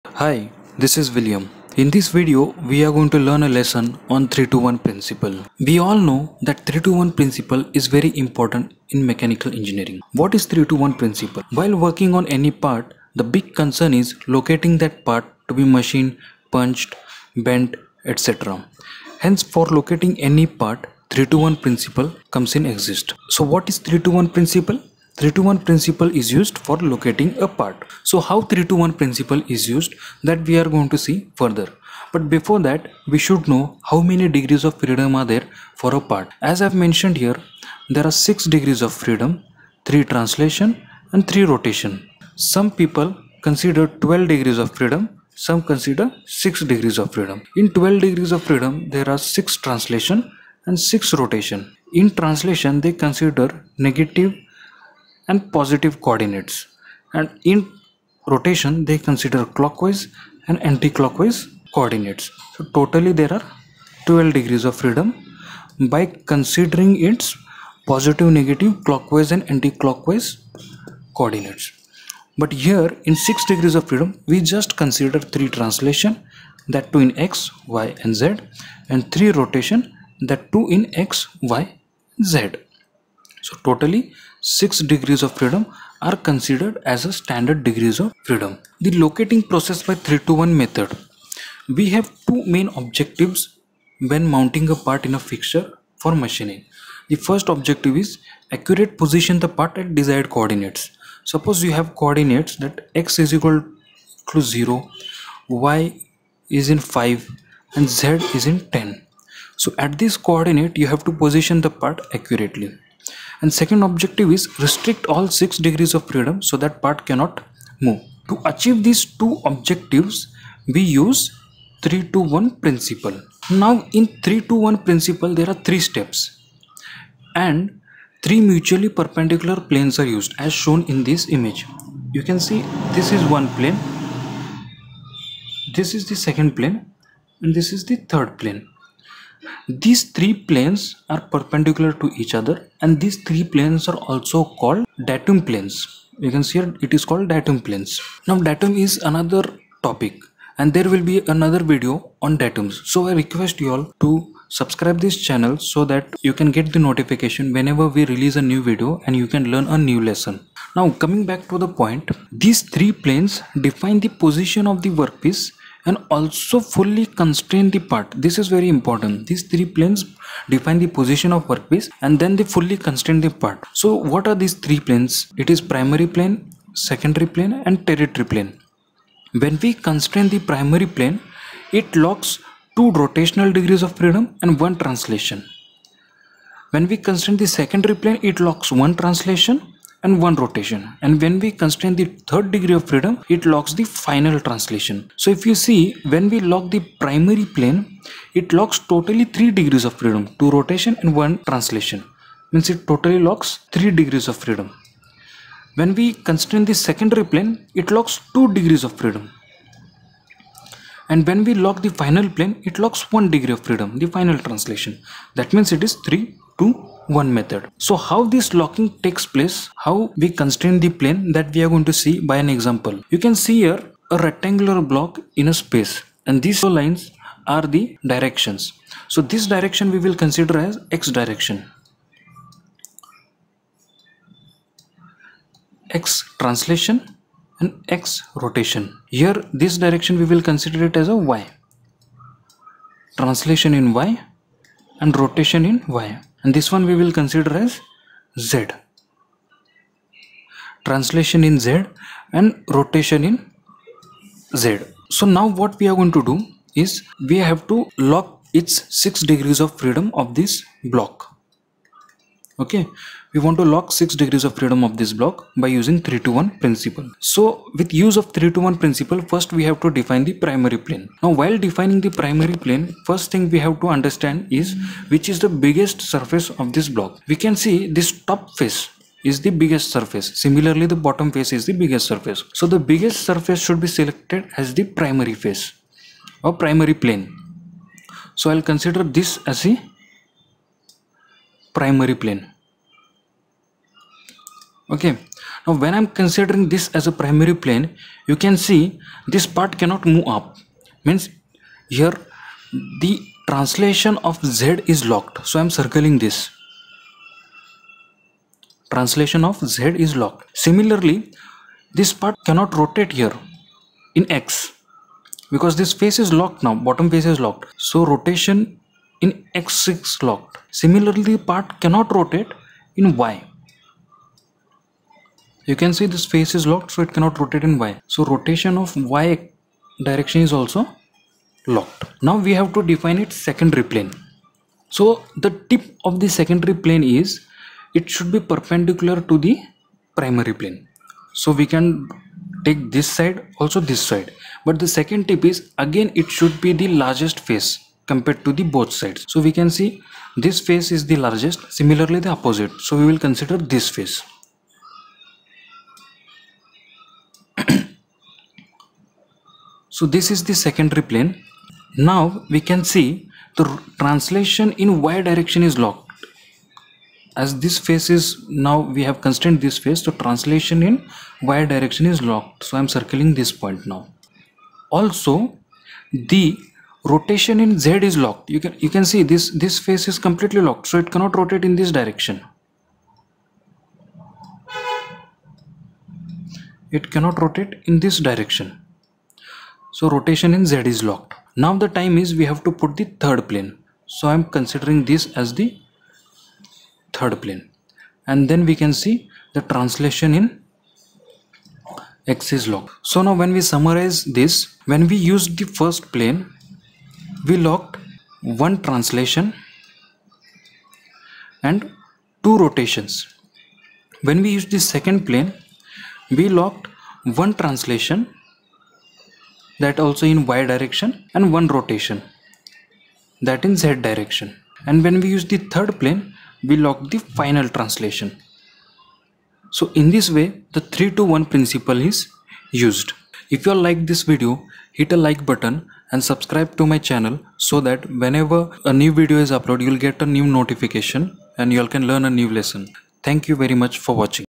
3-2-1 Principle of location | 3-2-1 Principle for fixture design | 6 Point Locating Principle
3-2-1 Principle is used for locating the part by locking or arresting its degree of freedom.
In this video you will get clarity on how to apply 321 principle for a part.
#321 principle
jigs and fixture